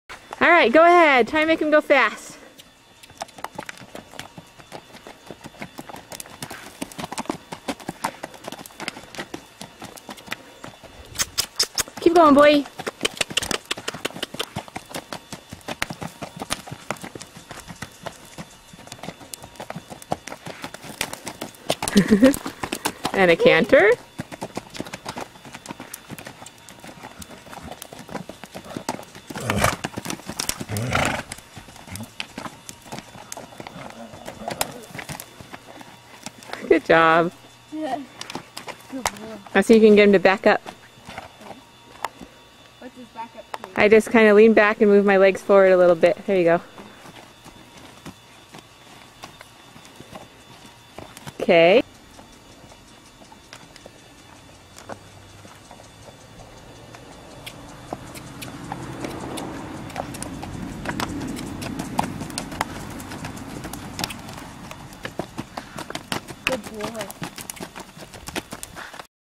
Mustang's Moonlight Night of Firsts with Tasha Jackson
Well, tonight I ran out for a minute at dusk to ride my stallion, Nozer (mustang's moonlight) bareback in a halter... I was helmetless (oh!!) and shoeless. and just taking it easy. It was my first time riding him in my sister's arena next door, because I have always just taken him on the trails. You can tell he doesn't understand what it means to stay on the rail and such. Then my 11 year old Niece, Tasha, came out and I offered to let her ride. She had never ridden bareback before, never ridden this horse before, and the only time she's ever ridden a gaited horse before was 2 years ago when I took her on a trail ride on my Tennessee walker I had then. I thought this video shows how naturally gaited Nozer is, considering his rider would have no idea how to keep him in a gait (doesn't even know what its supposed to feel like). It also shows was a great rider my niece is! Good Job Tasha!

Love ya!